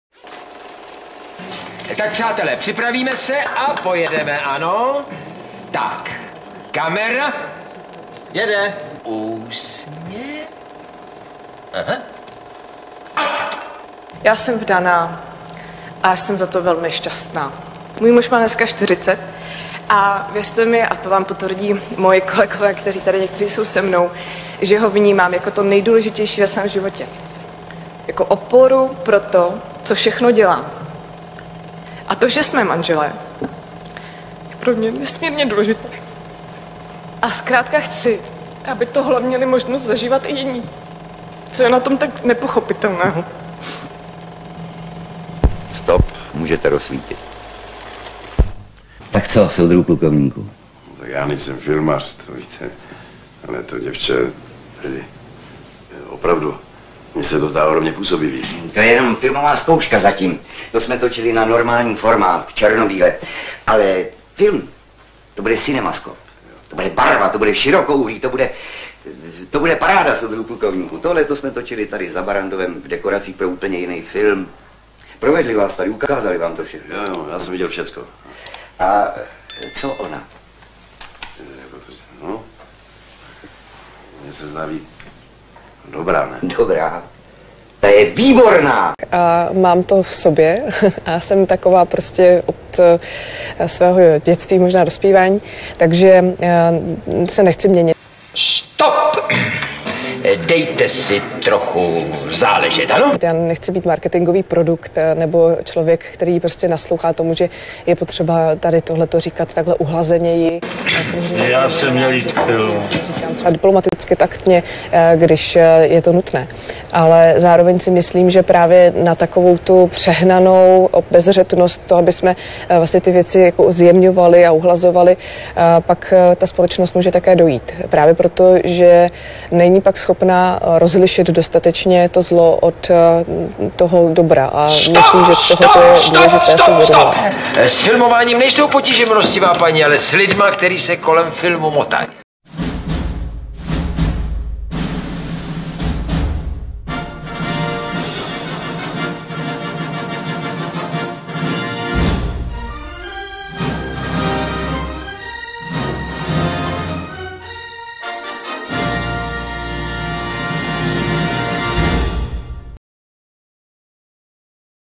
Předsedkyně Poslanecké sněmovny Adamová Pekarová děla díru do světa showbyznysu
Pokud máte zájem podpořit tento kanál, můžete na čísle účtu: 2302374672/2010
Děkuji!

✔️ Sledujte Český List